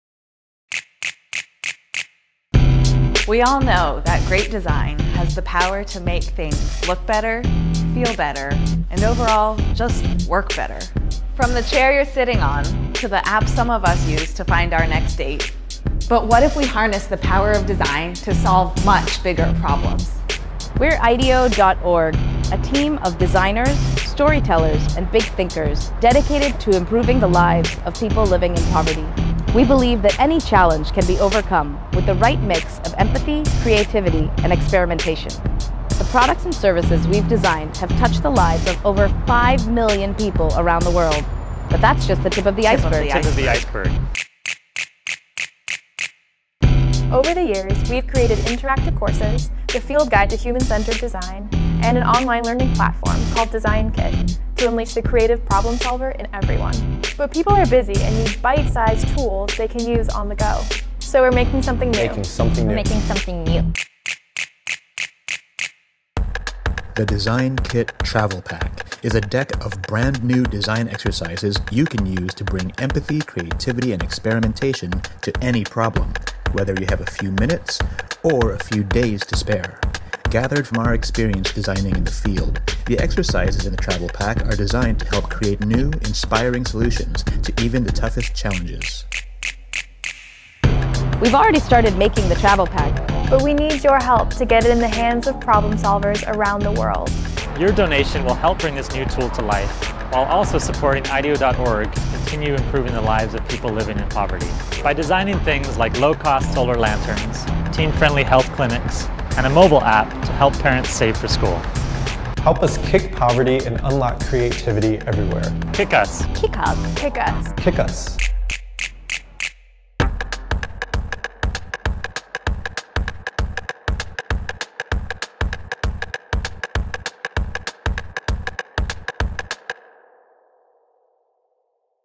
Introducing Design Kit: Travel Pack
The Travel Pack is a deck of bite-sized design exercises to bring empathy, creativity, and experimentation to any challenge. Available exclusively on Kickstarter until December 22: kck.st/2Bgkuvs